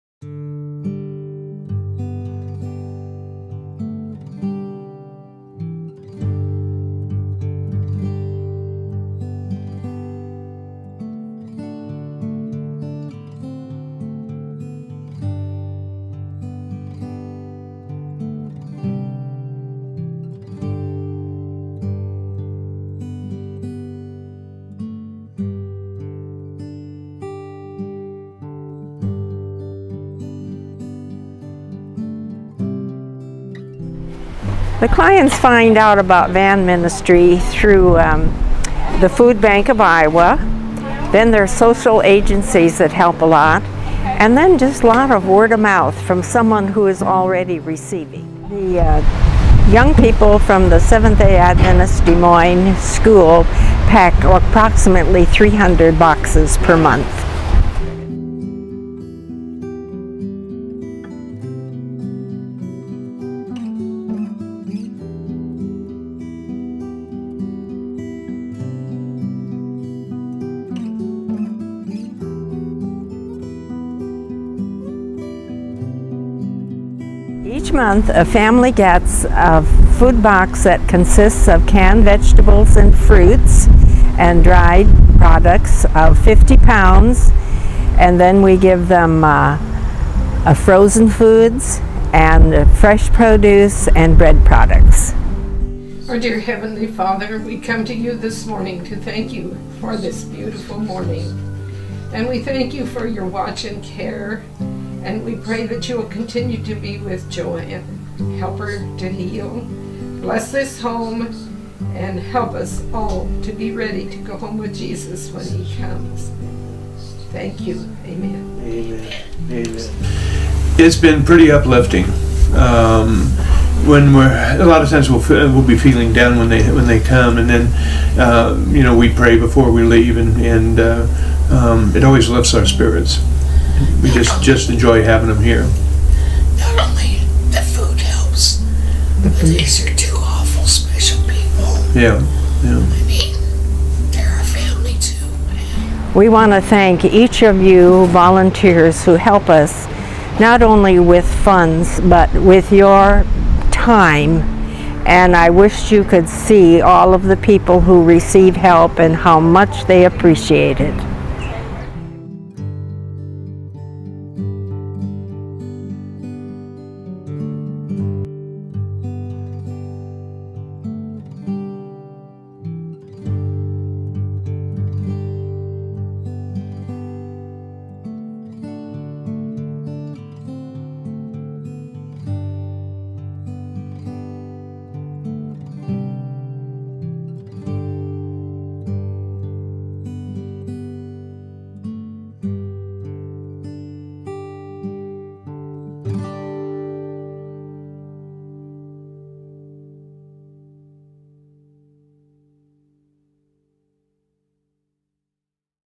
Van Ministry Promo 2012
This video is about the Des Moines Area Van Ministry, an Adventist Community Services Outreach.